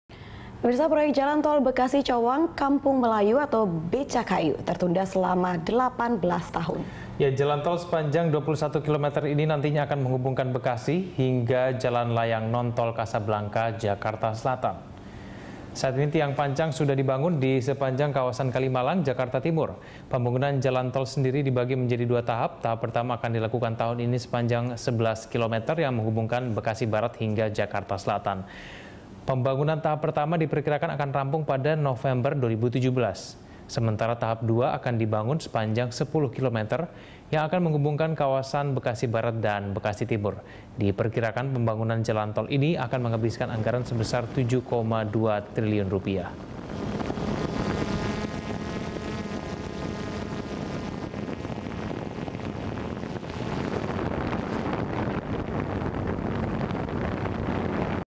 Sempat Tertunda 18 Tahun, Proyek Tol Becakayu kembali Dibangun
Proyek jalan Tol Bekasi, Cawang, Kampung Melayu atau Becakayu yang sempat tertunda selama 18 tahun kini dibangun kembali.

Facebook.com/BeritaSatuTV
Youtube.com/BeritaSatu